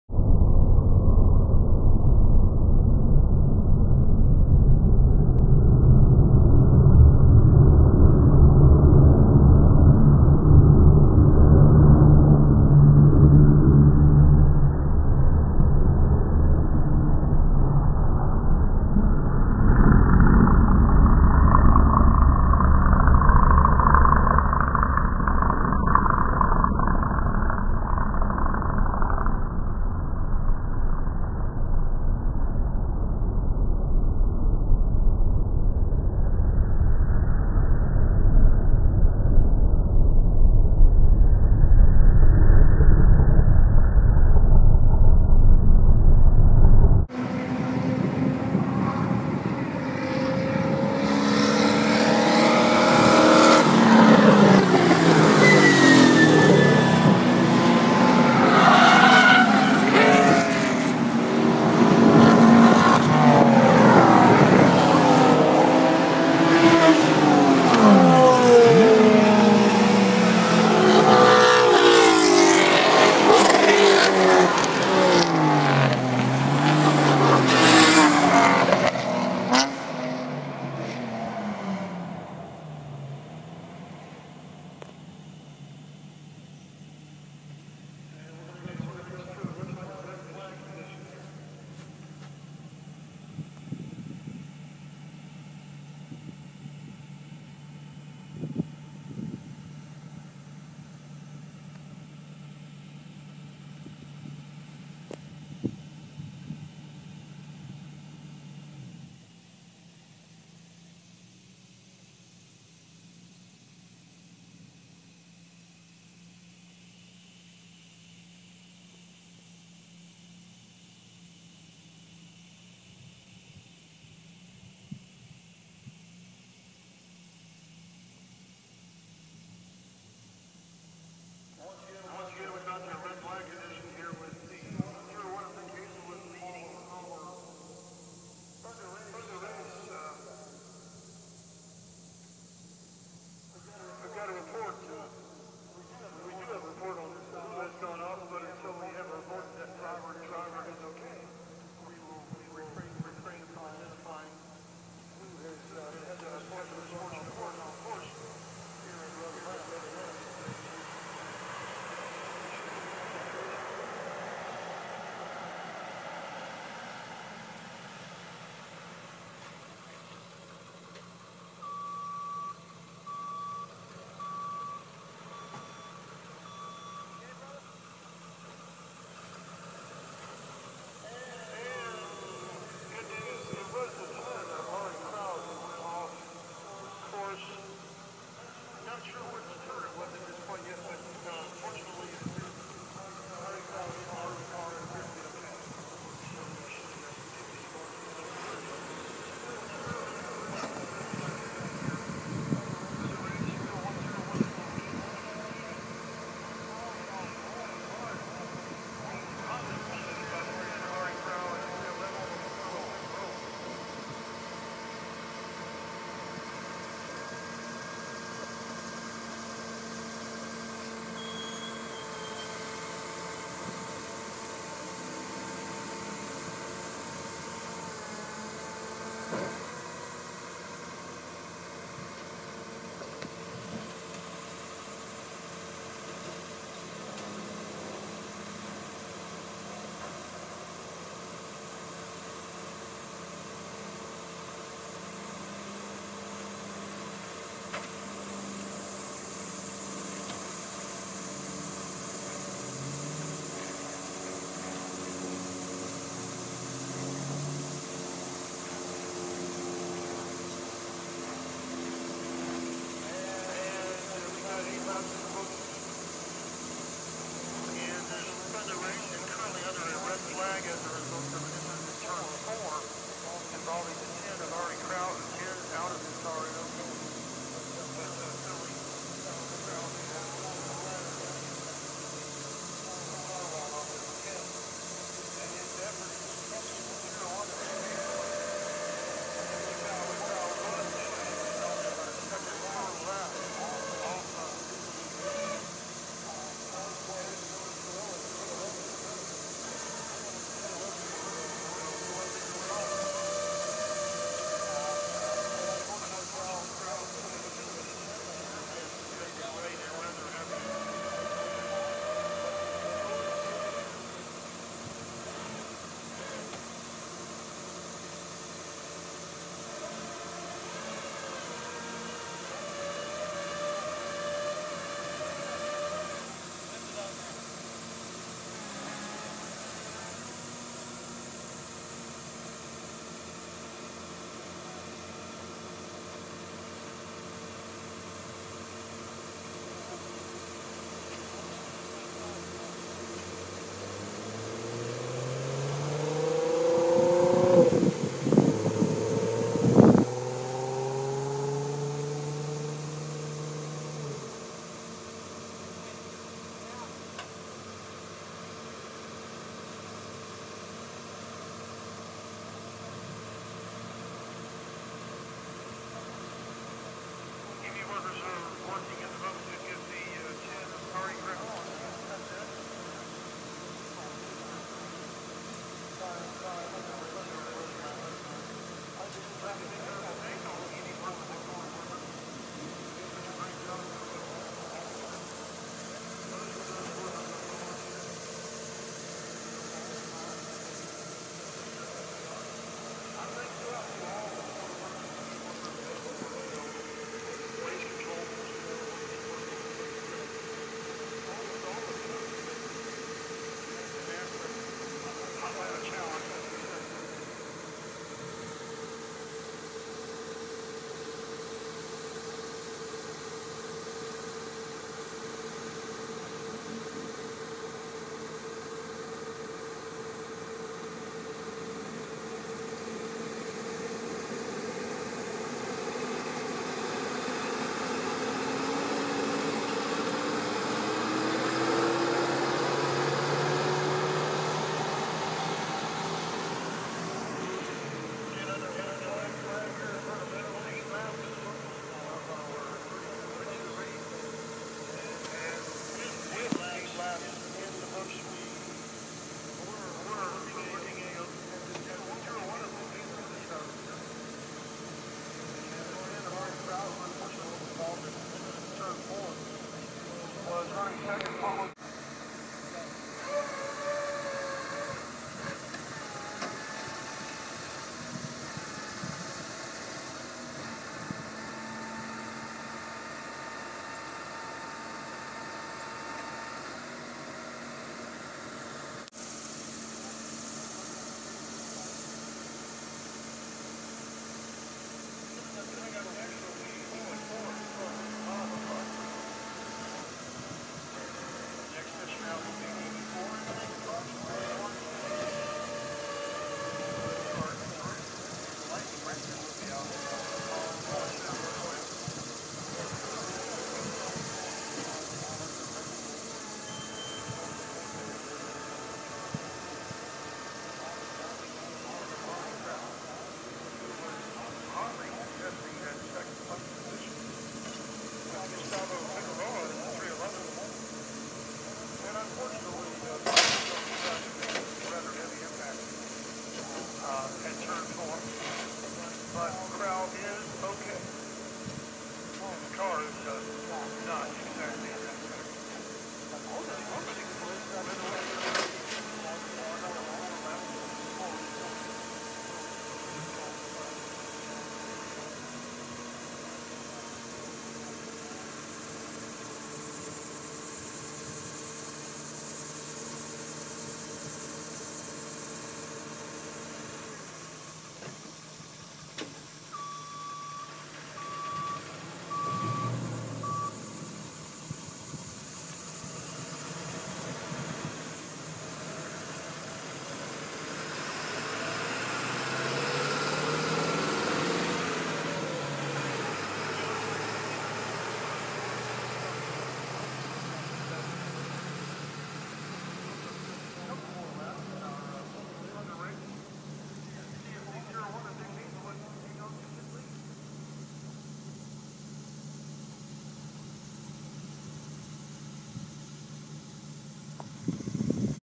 Road Atlanta Chevy Corvette Z06 Crash @ NASA 2012
Jeff Buckner's 2002 Z06 #10 Corvette crash on Sunday August 5th 2012 at Road Atlanta for the NASA Hotlanta Challenge. The driver was ok. Immediately after the crash there was a burst of fire under the car that I don't think you can see in the video. Sorry for the shaky and not zoomed in video but I was using an Iphone and those aren't the best cameras for the esses. Enjoy!